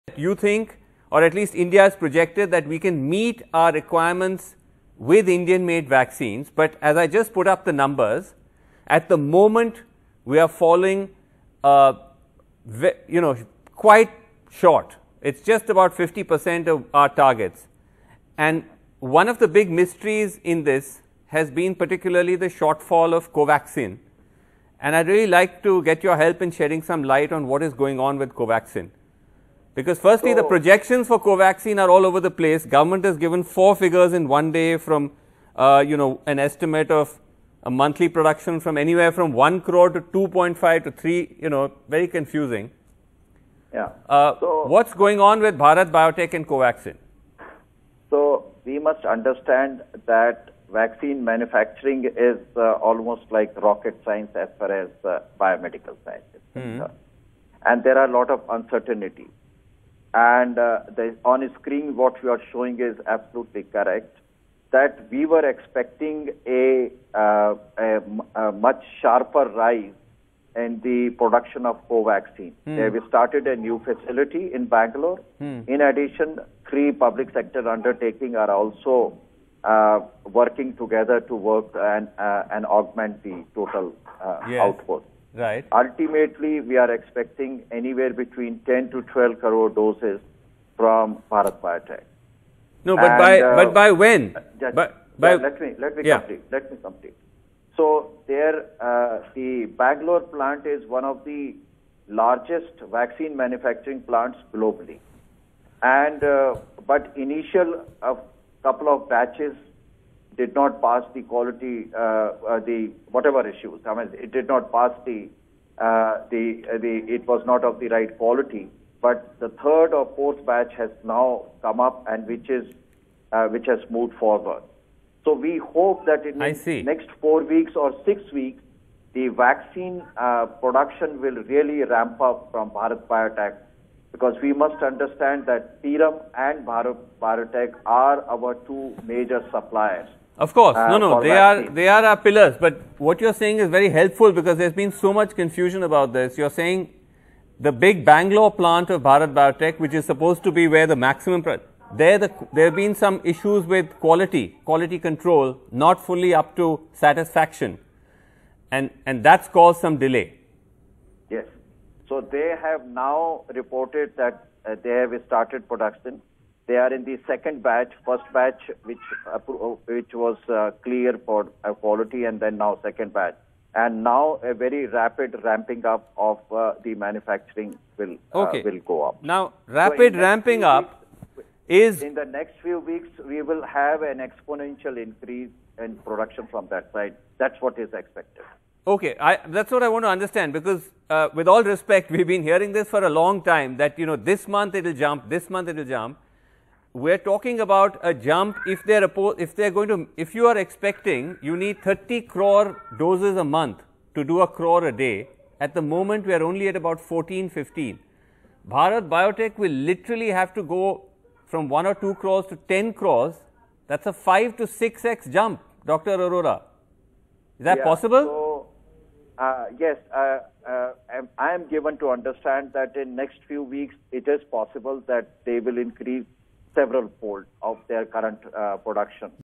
Some Covaxin Batches Were Not Released Due To Quality Issues: Official | Reality Check
The supply of Bharat Biotech's Covaxin has been slowed down because the first few batches at the company's newest facility in Bengaluru were not of the right quality, said Dr NK Arora, head of the vaccine advisory panel.